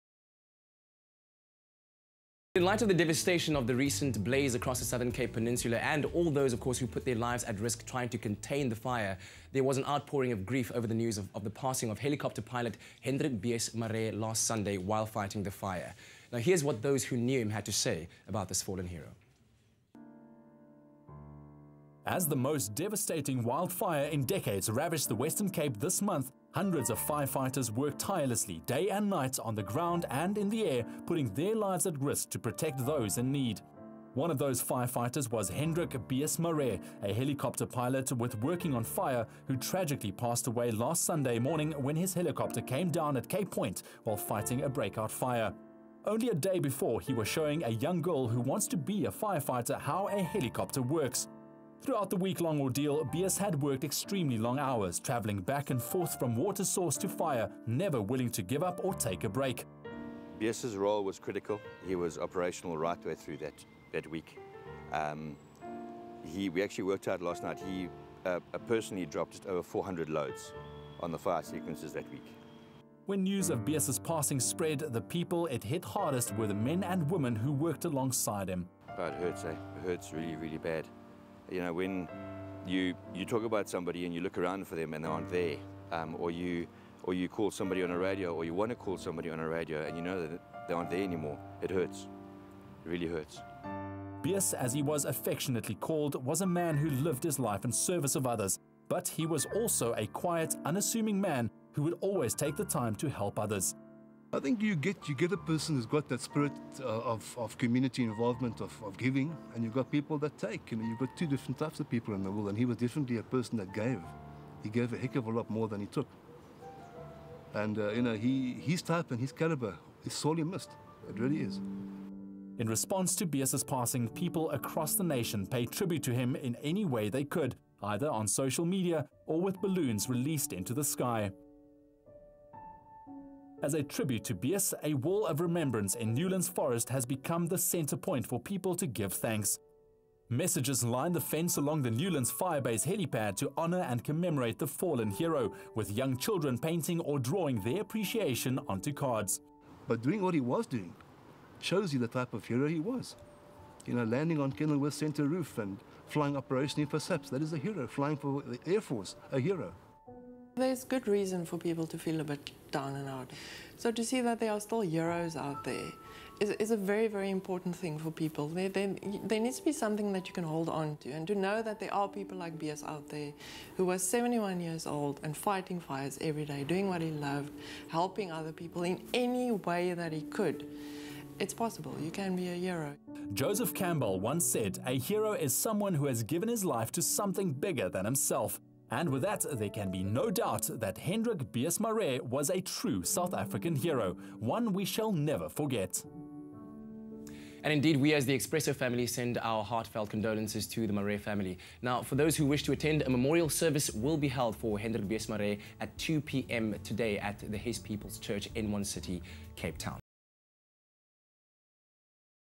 Tribute to a fallen hero, Hendrik “Bees” Marais
As the most devastating wildfire in decades ravaged the Western Cape this month, hundreds of firefighters worked tirelessly, day & night - on the ground & in the air, putting their lives at risk to protect those in need.

One of those firefighters was Hendrik “Bees” Marais, a helicopter pilot with Working On Fire, who tragically passed away last Sunday morning when his helicopter came down at Cape Point while fighting a break-out fire.